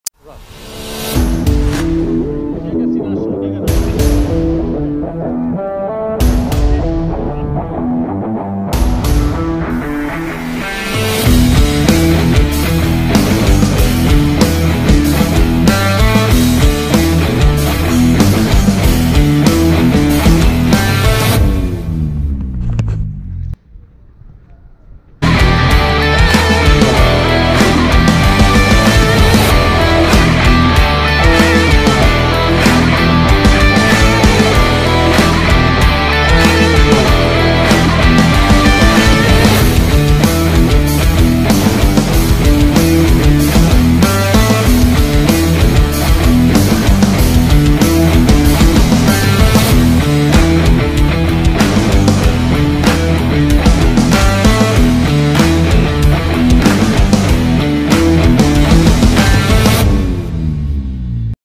Penalty Challenge - FK Voždovac - Nišić vs. Knežević
PenaltyChallenge na krovu!

Nišić je šutirao, a Marko Knežević branio 🧤⚽

Pogledajte video i saznajte ko je bio spretniji